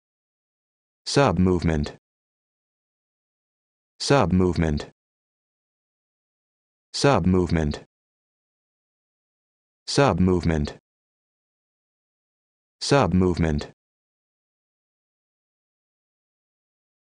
How to Pronounce Submovement
This video shows you how to pronounce Submovement